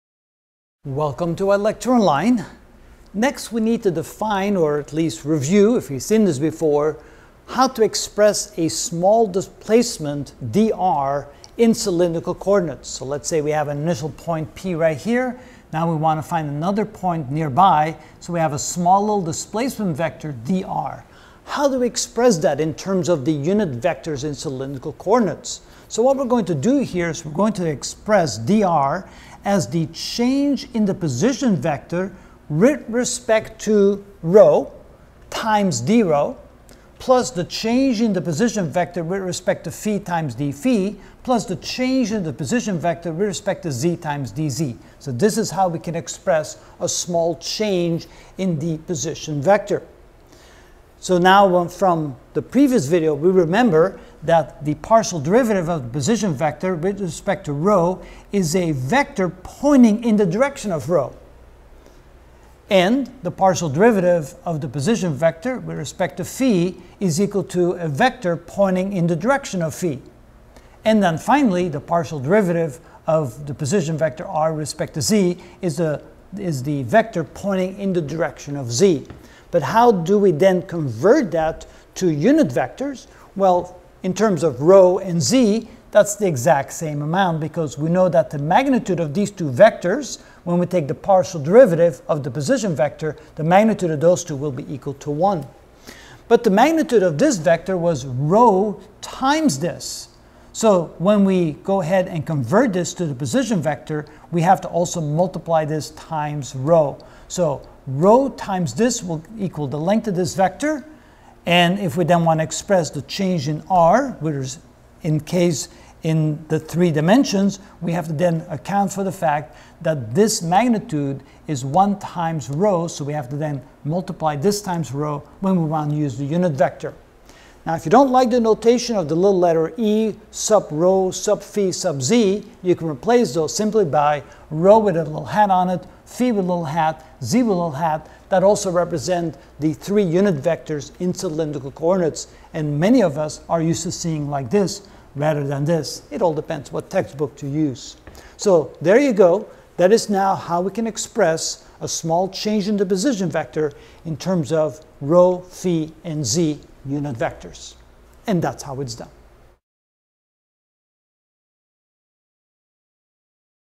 Calculus 3: Divergence and Curl (34 of 50) Cylindrical Coordinates: Small Displacement dr
In this video I will define the dr in cylindrical coordinates.